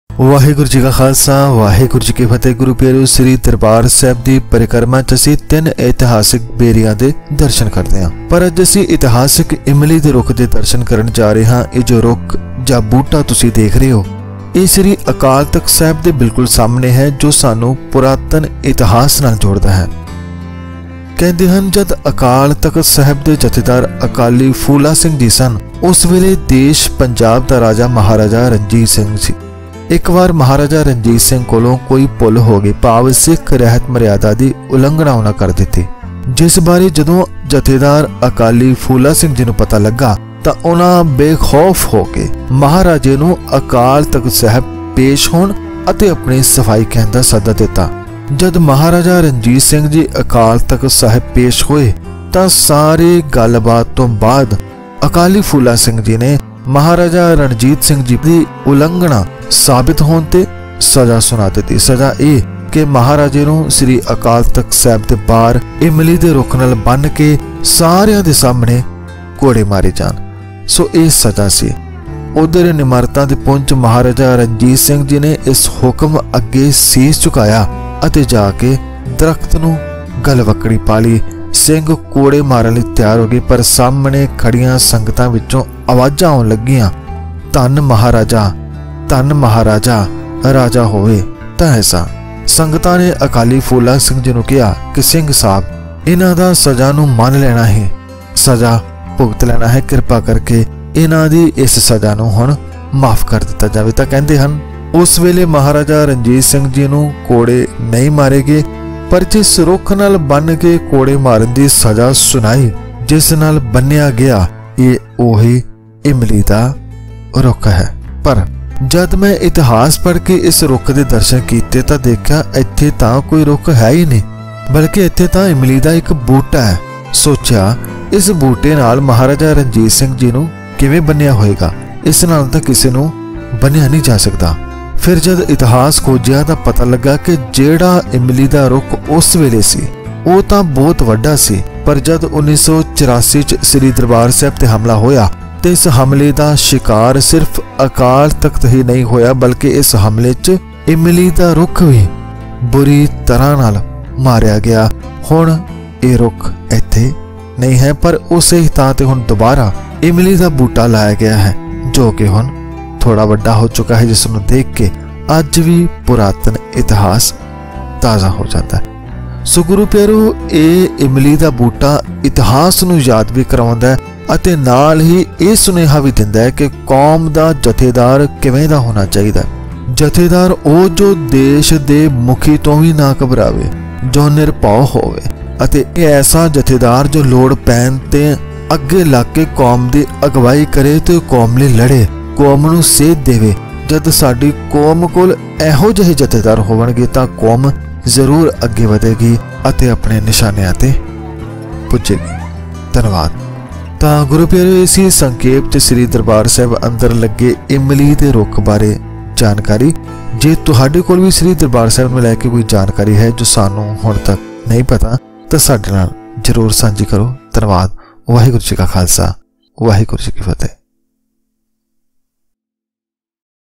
PART 8 - DARBAR SAHIB - ਇਤਿਹਾਸਿਕ ਇਮਲੀ ਦੇ ਰੁੱਖ ਦੀ ਕਹਾਣੀ
ਸ੍ਰੀ ਦਰਬਾਰ ਸਾਹਿਬ ਬਾਰੇ ਰੋਚਕ ਤੱਥ Facts About Sri Darbar Sahib

ਅਕਾਲ ਤਖ਼ਤ ਦੇ ਸਾਮਣੇ ਹੈ, ਇੱਕ ਇਮਲੀ ਦੀ ਰੁੱਖ, ਜਿਸ ਦਾ ਇਤਿਹਾਸ ਜੁੜਿਆ ਹੈ, ਅਕਾਲੀ ਫੂਲਾ ਸਿੰਘ ਜੀ ਅਤੇ ਮਹਾਰਾਜਾ ਰਣਜੀਤ ਸਿੰਘ ਜੀ ਦੇ ਨਾਲ Historical Tree At SriDarbarSahib